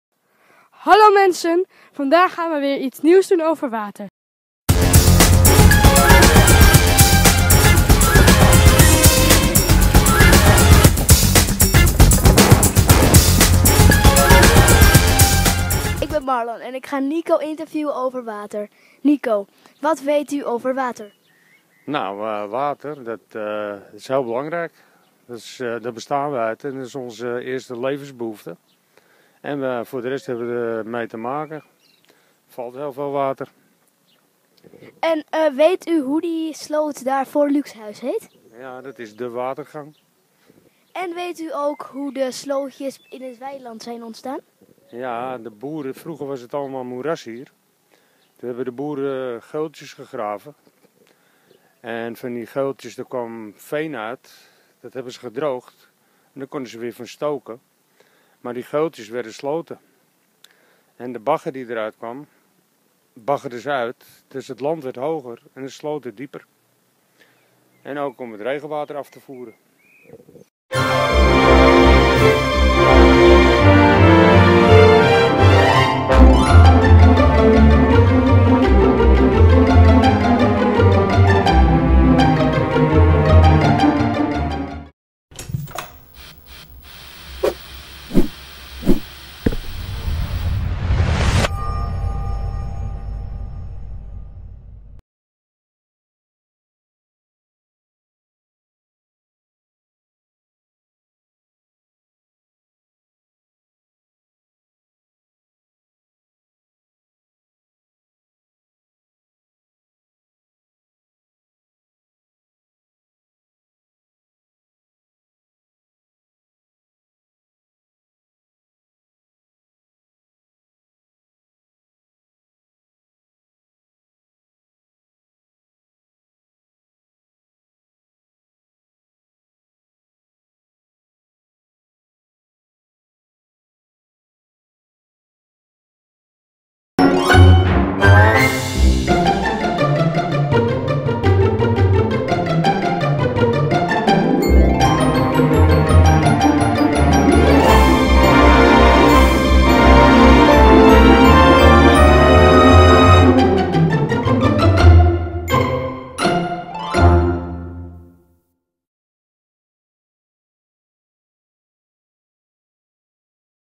De frisse blik op 'Water' van Luuk en Marlon
Films gemaakt door leerlingen van basisschool de Van Randwijk school te Ilpendam, tijdens het media-erfgoed project ʻWaterʼ. Gedurende het project gaan leerlingen van vijf bassisscholen uit de regioʼs Waterland en de Beemster op zoek naar het historisch erfgoed rond ʻwaterʼ, met behulp van nieuwe media. Leerlingen gaan, bemand met iPads, op pad om ﬁlms te maken over de historie van Waterland aan de hand van hedendaagse verhalen, historische voorwerpen en plekken waar het verleden zichtbaar blijkt te zijn. Mediawijsheidsprojecten in samenwerking met stichting De frisse blik.